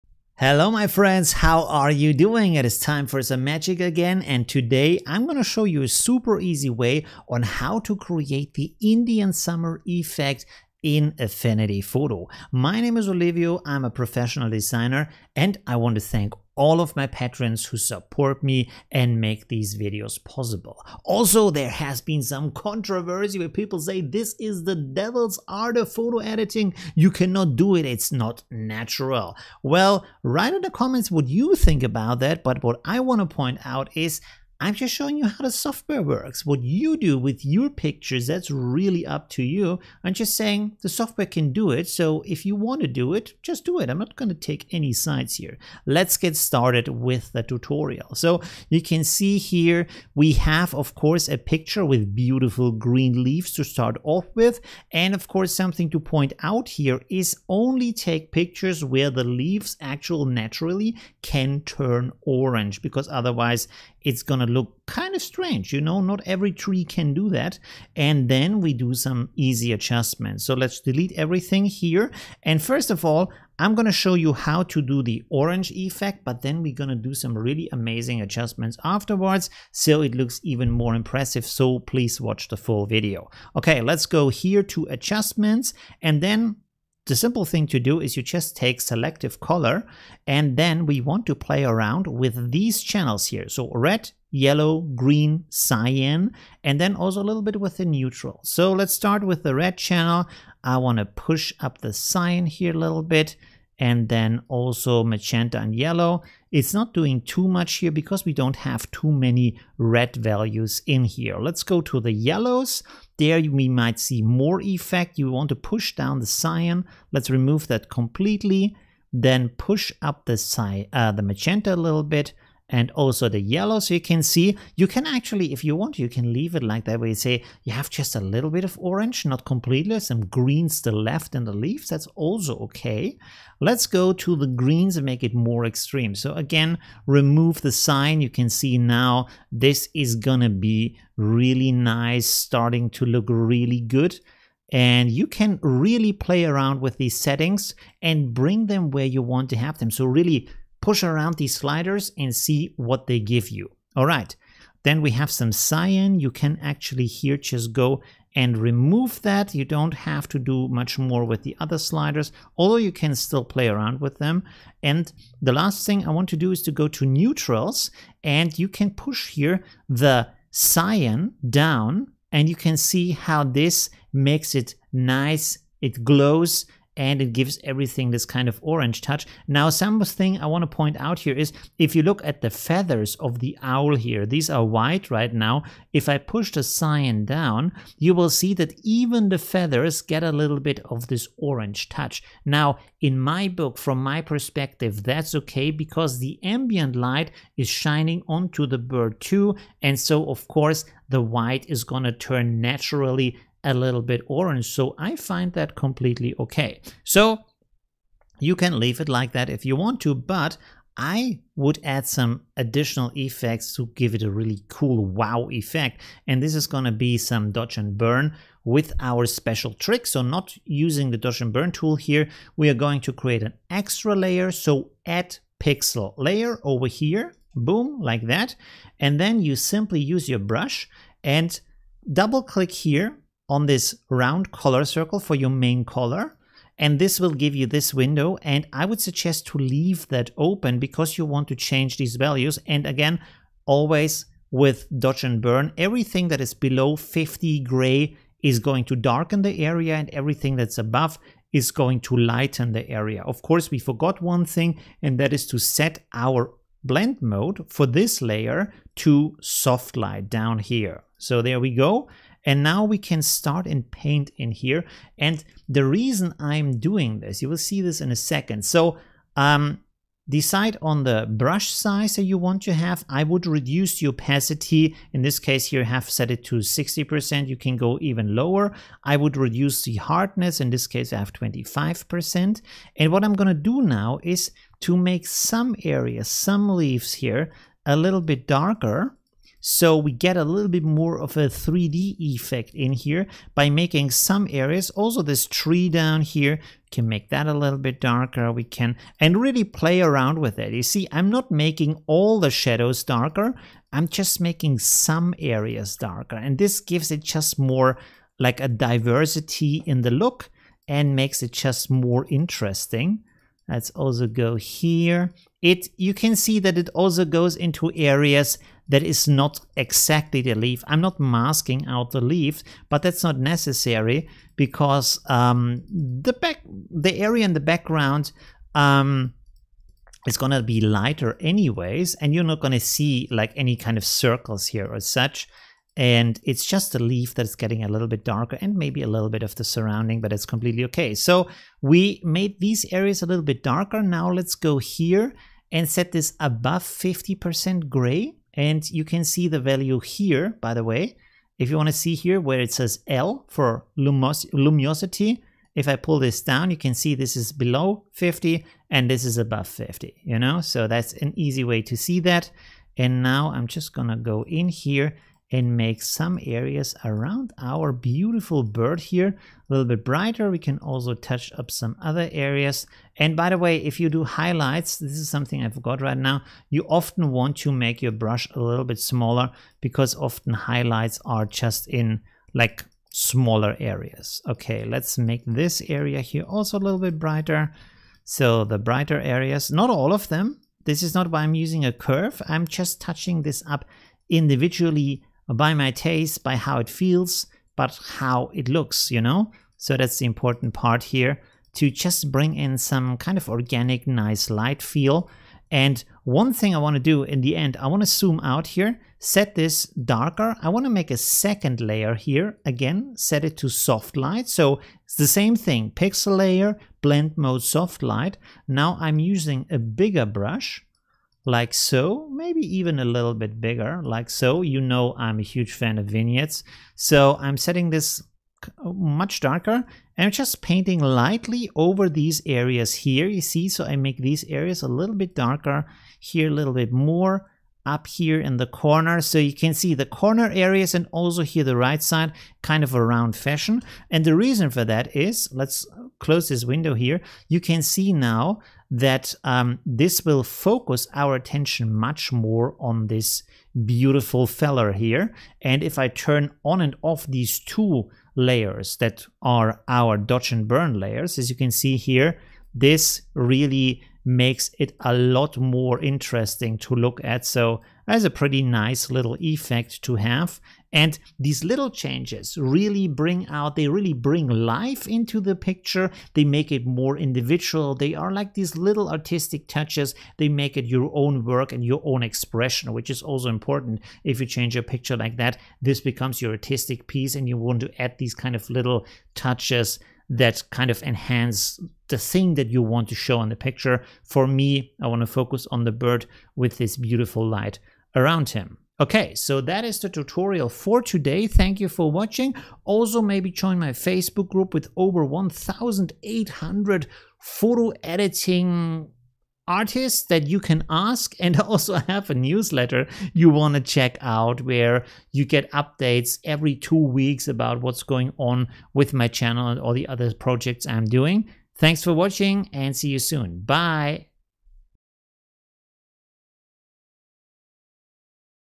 Indian Summer Effect - Affinity Photo Tutorial for Beginners
Watch this easy Indian Summer Effect Tutorial for Affinity Photo. With just 2 simple adjustments, you can turn your photos into Autumn Magic. Beautiful orange leaves, nice glowing light and a hand painted light & shadow atmosphere to impress. This Affinity Photo Tutorial is perfect for Beginners